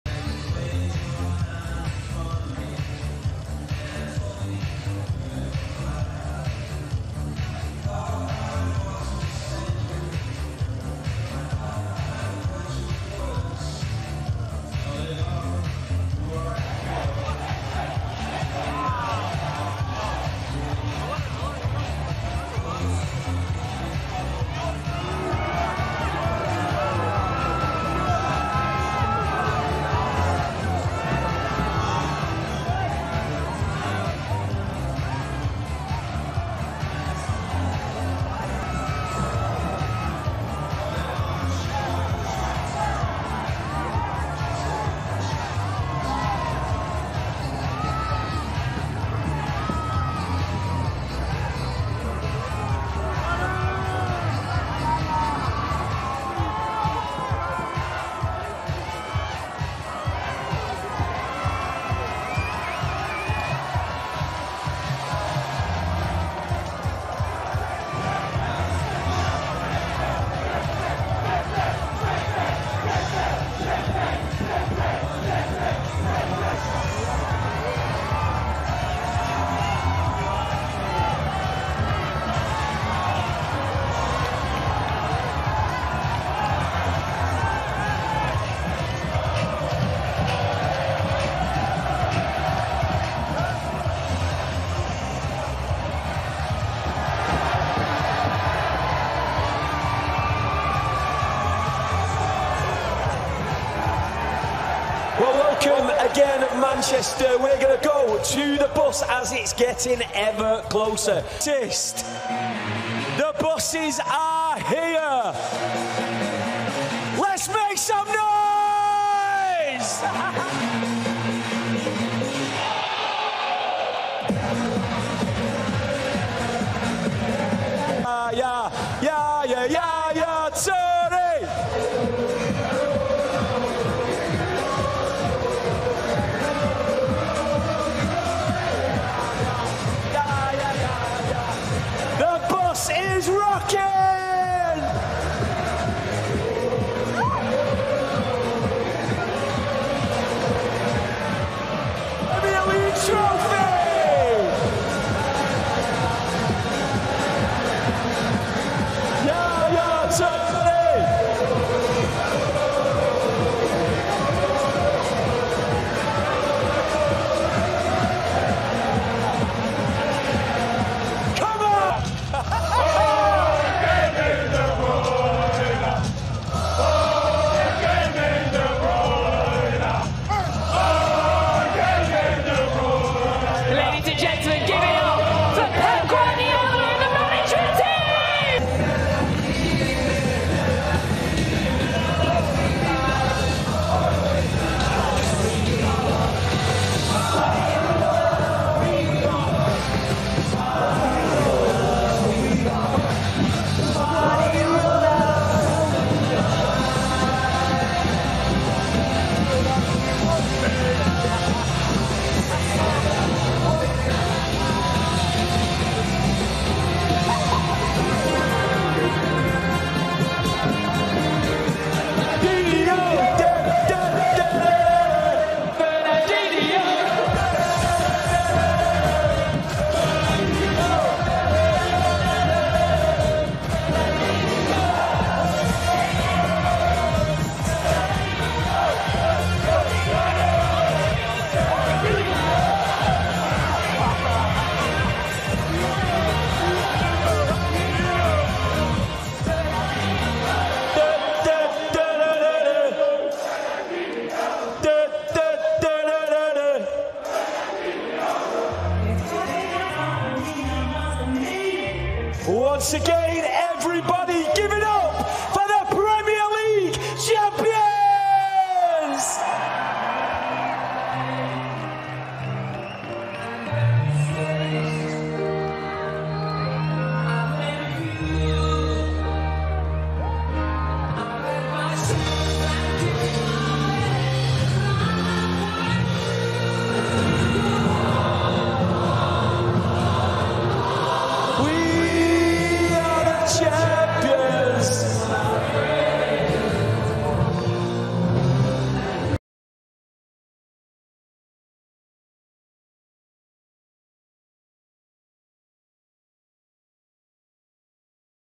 Champions Manchester City paint town blue with open top bus parade
JACK GREALISH drank beers and Pep Guardiola smoked on a fat cigar as Man City celebrated their Premier League title win with an open-top bus parade through Manchester city centre.

Pep and Co sealed a fourth Prem trophy in the last five seasons after his side completed a miraculous comeback against Aston Villa - coming from two goals behind to win 3-2.